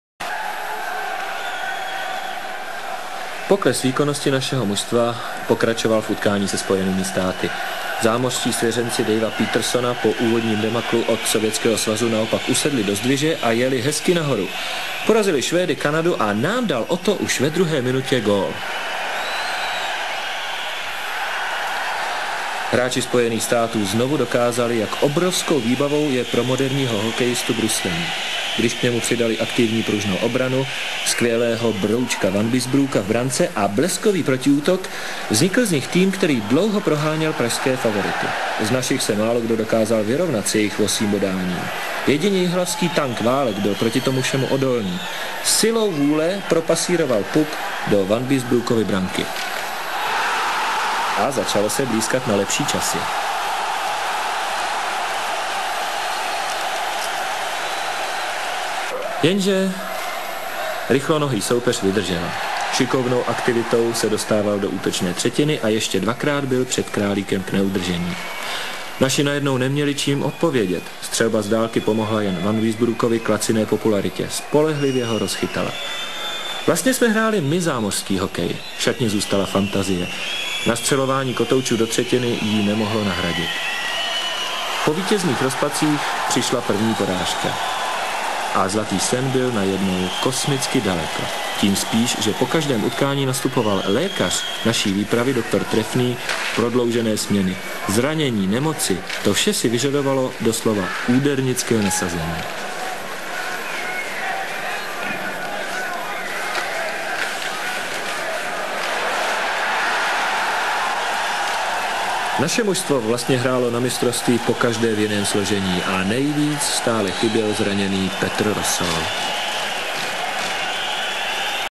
IIHF WC 1985 Czechoslovakia - USA 1-3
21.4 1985
Branky: 32:00 Válek (Pivoňka) -- 1:57 Otto (R. Miller), 37:52 Granato (K. Miller), 57:44 Millen.

ČSSR:   Králík -- Benák, Uvíra, A. Kadlec, Hořava, Stavjaňa, F. Musil -- V. Lukáč, Rusnák, Liba -- Hrdina, Růžička, Richter -- Lála, Pašek, Šejba -- Válek, Kameš, Pivoňka.

USA:   Vanbiesbrouck -- Mantha, J. Johnson, O'Connell, Suter, Sheehy, Fusco -- Fenton, Millen, Thomas -- Brooke, M. Johnson, A. Broten -- Fergus, Otto, R. Miller -- Donatelli, Granato, K. Miller.